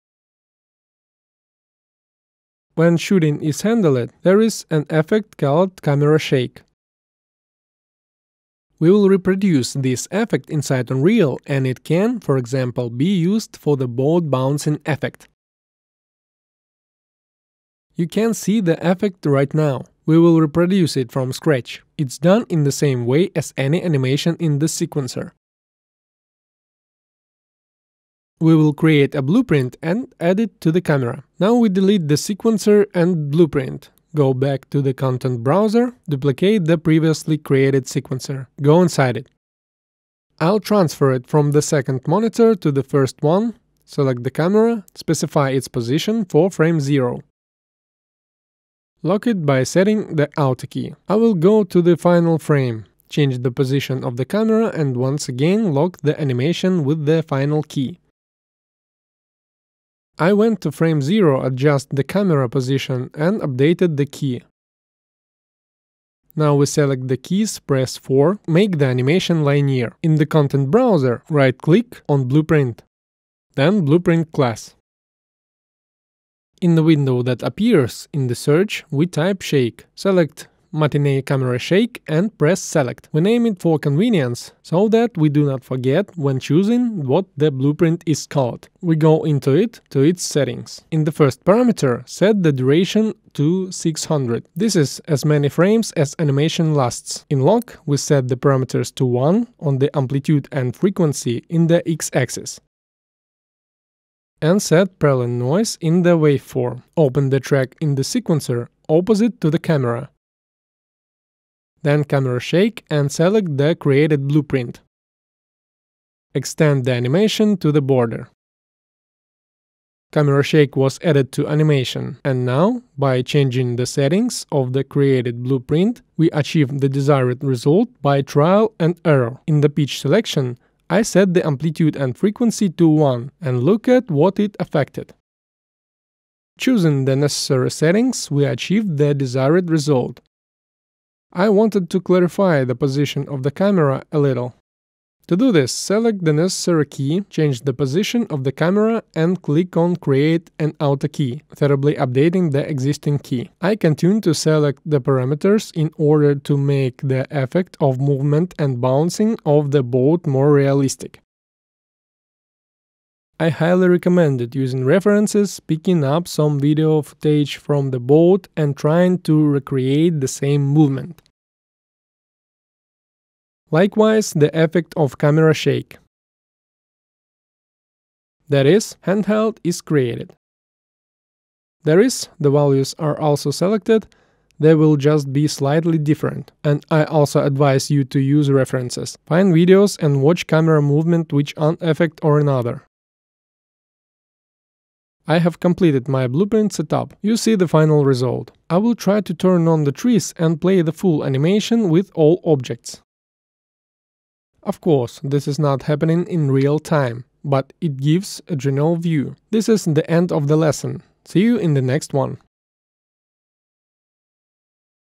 How to make camera shake in Unreal Engine 5 | Exterior in Unreal 5
In this video I'm going to show how to make camera shake in Unreal Engine 5

My laptop: Acer Predator GX-792-78YD